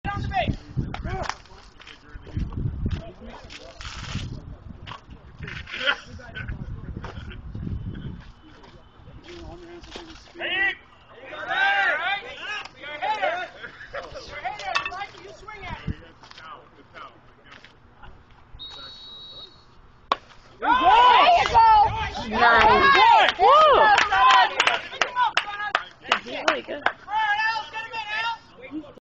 Grays vs Ballers_game2.mpeg
SASL - Fall League
Date: Sept. 26, 2010
Game #2 -- Grays vs. Ballers
Sanat Patel lines a hit to LF